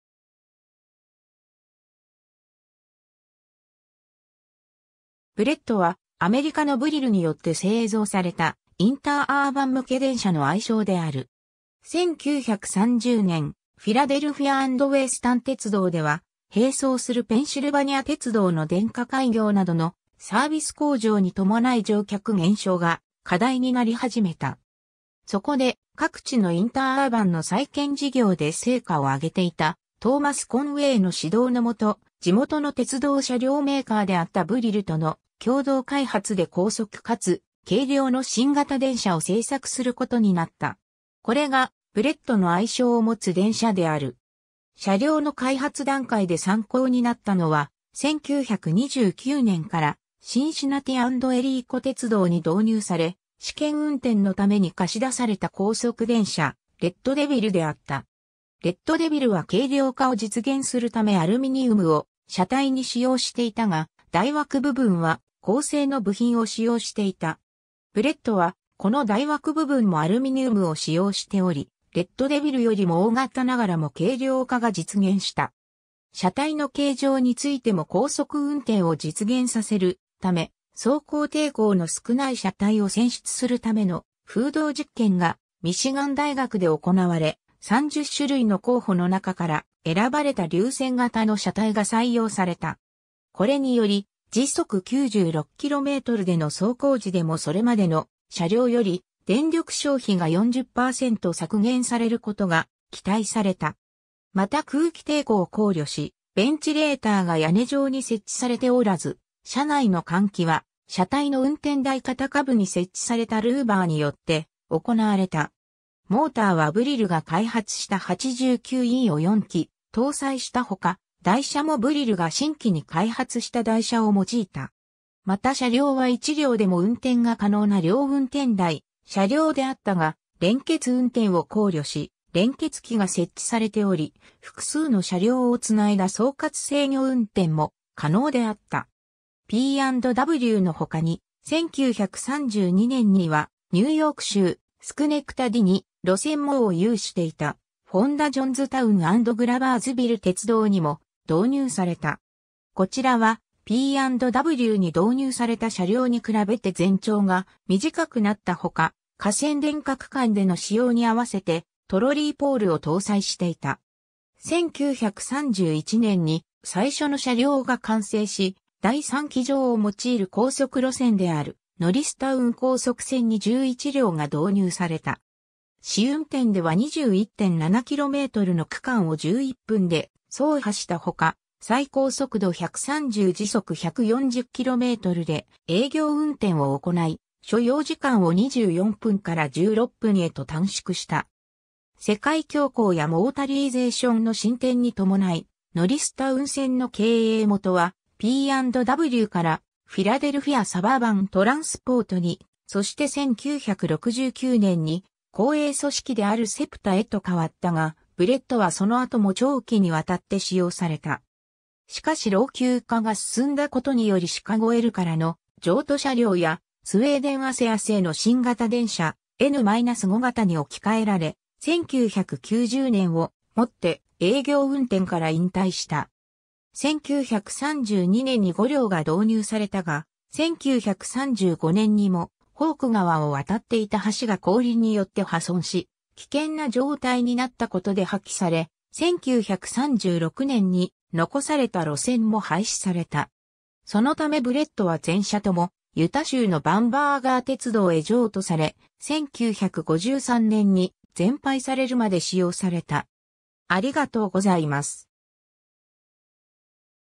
ブレット (鉄道車両)
ブレット（英語: Bullet）は、アメリカのブリルによって製造された、インターアーバン向け電車の愛称である。
 1930年、フィラデルフィア・アンド・ウェスタン鉄道（P&W, Philadelphia and Western Railroad）では、並走するペンシルバニア鉄道の電化開業などのサービス向上に伴い乗客減少が課題になり始めた。
そこで、各地のインターアーバンの再建事業で成果をあげていたトーマス・コンウェイ（Thomas Conway Jr.）の指導の下、地元の鉄道車両メーカーであったブリルとの共同開発で高速かつ軽量の新型電車を製作する事になった。
これがブレットの愛称を持つ電車である。
 車両の開発段階で参考になったのは、1929年からシンシナティ・アンド・エリー湖鉄道（C&LE, Cincinnati and Lake Erie Railroad）に導入され、試験運転のために貸し出された高速電車・レッドデビルであった。
レッドデビルは軽量化を実現するためアルミニウムを車体に使用していたが、台枠部分は鋼製の部品を使用していた。
ブレットはこの台枠部分もアルミニウムを使用しており、レッドデビルよりも大型ながらも軽量化が実現した。
 車体の形状についても高速運転を実現させるため、走行抵抗の少ない車体を選出するための風洞実験がミシガン大学で行われ、30種類の候補の中から選ばれた流線型の車体が採用された。
これにより、96km/hでの走行時でもそれまでの車両より電力消費が40％削減される事が期待された。
また空気抵抗を考慮しベンチレーターが屋根上に設置されておらず、車内の換気は車体の運転台方下部に設置されたルーバーによって行われた。
 モーターはブリルが開発した89-E（100馬力）を4基搭載した他、台車もブリルが新規に開発した台車を用いた。
また車両は1両でも運転が可能な両運転台車両であったが、連結運転を考慮し連結器が設置されており、複数の車両を繋いだ総括制御運転も可能であった。
 P&Wの他に、1932年にはニューヨーク州・スクネクタディに路線網を有していたフォンダ・ジョンズタウン・アンド・グラバーズビル鉄道（FJ&G, Fonda, Johnstown and Gloversville Railroad）にも導入された。
こちらはP&Wに導入された車両に比べて全長が短くなった他、架線電化区間での使用に合わせてトロリーポールを搭載していた。
 1931年に最初の車両が完成し、第三軌条を用いる高速路線であるノリスタウン高速線に11両が導入された。
試運転では21.7km（13.5マイル）の区間を11分で走破した他、最高速度130～140km/hで営業運転を行い、所要時間を24分から16分へと短縮した。
 世界恐慌やモータリーゼーションの進展に伴い、ノリスタウン線の経営元はP&Wからフィラデルフィア・サバーバン・トランスポートに、そして1969年に公営組織であるSEPTA（南東ペンシルベニア交通局）へと変わったが、ブレットはその後も長期に渡って使用された。
しかし老朽化が進んだ事によりシカゴ・Lからの譲渡車両やスウェーデン・アセア製の新型電車・N-5形に置き換えられ、1990年をもって営業運転から引退した。
 1932年に5両が導入されたが、1935年にモホーク川を渡っていた橋が氷によって破損し危険な状態になった事で破棄され、1936年に残された路線も廃止された。
そのためブレットは全車ともユタ州のバンバーガー鉄道（Bamberger Railroad）へ譲渡され、1953年に全廃されるまで使用された。